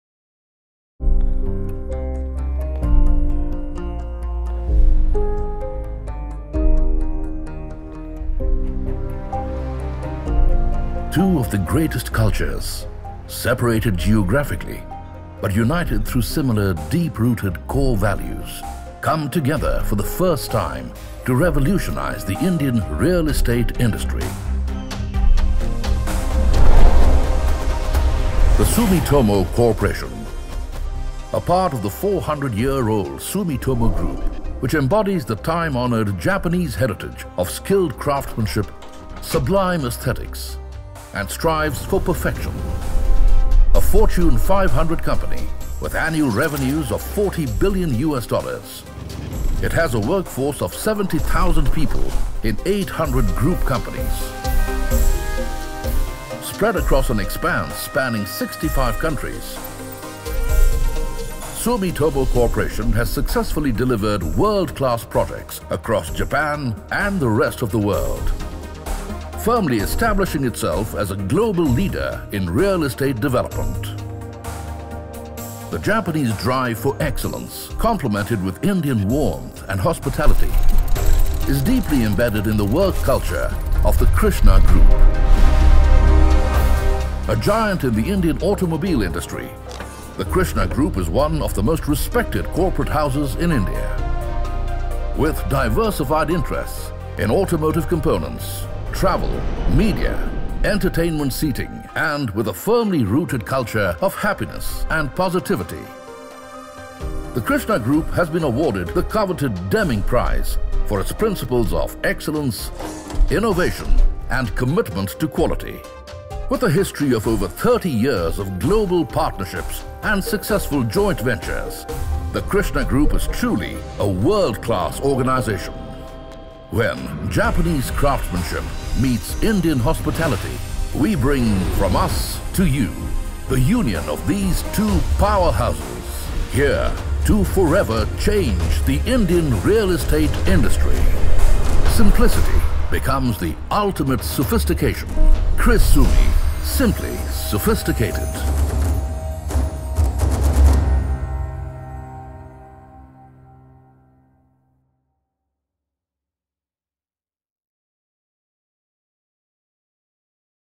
Krisumi Corporate Film
An enterprising collaboration between Sumitomo Corporation, Japan and Krishna Group, India, Krisumi is a convergence of Kriya, meaning creation in Sanskrit, and Sumi, the Japanese word for fine living.

Our endeavour is to conceptualize, design and build homes which are characterized by the highest standards of Japanese craftsmanship, in sizes that complement today’s ever-evolving way of life. By unifying Japanese design aesthetics with the warmth of Indian hospitality, we at Krisumi are giving shape to architectural masterpieces that are spectacular in form, and highly practical in function.

Driven by a 400–year legacy of excellence and trust, and a commitment to deploying cutting-edge design and technology, Krisumi is where Sophistication is Simplified.